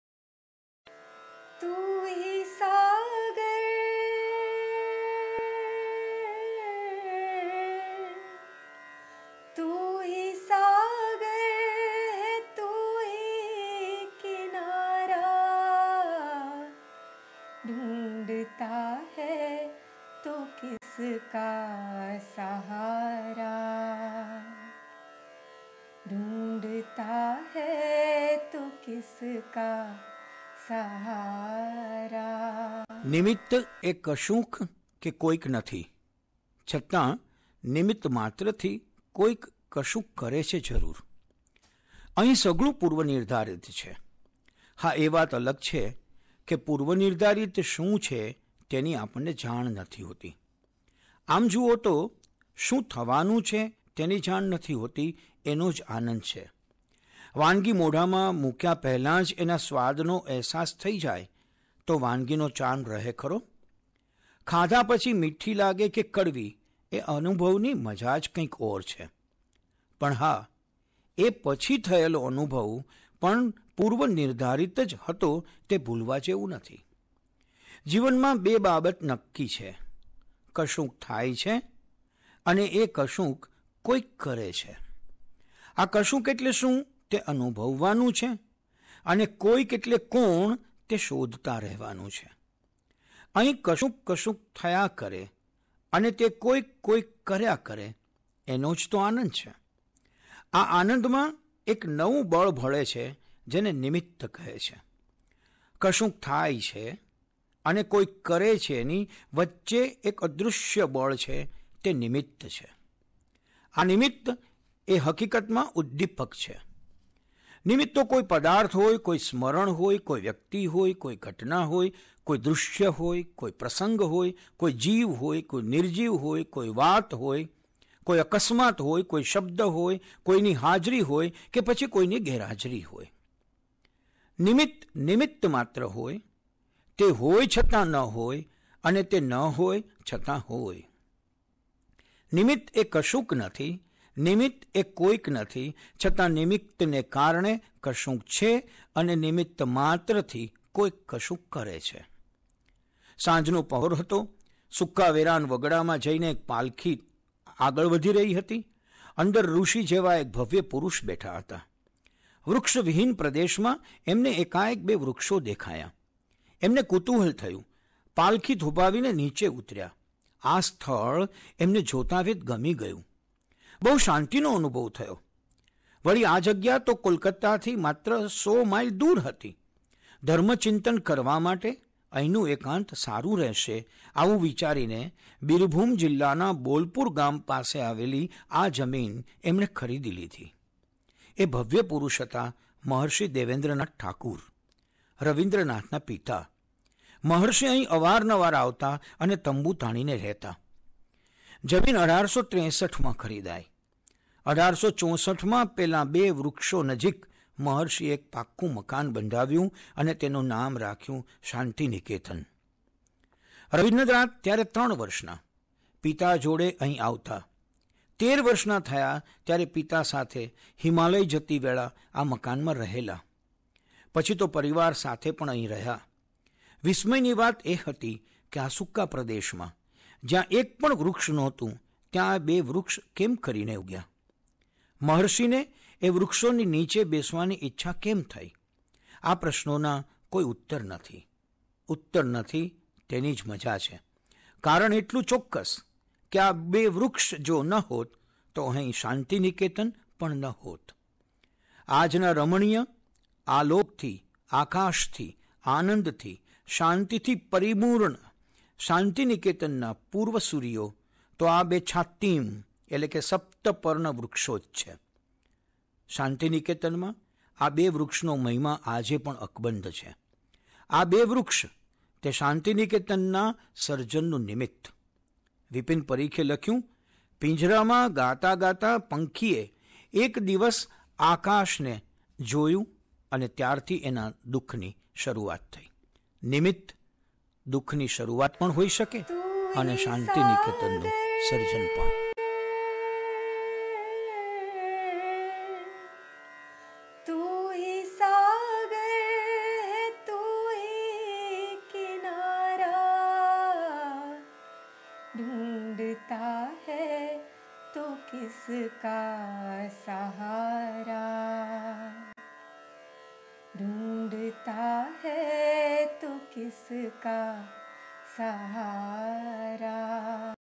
|| મંગળ મંથન : ૫૫ || નિમિત્ત એટલે શું ?
વિચારની વસાહતો તરફની યાત્રા:

::: "નિમિત્ત એટલે શું ? " :::
(પ્રતિ મંગળવાર : એક વિચાર-મંથન) લેખન+પ્રસ્તુતિ:
ભદ્રાયુ વછરાજાની

Bhadrayu Vachhrajani